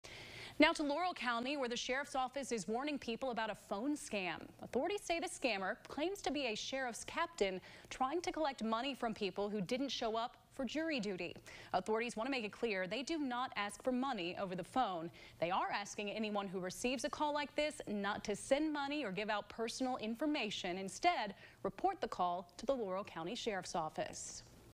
Jury duty scam circulating in Laurel County
Sheriff John Root said that the caller identifies themselves as a sheriff’s captain and urges residents to pay a fee after failing to report for jury duty.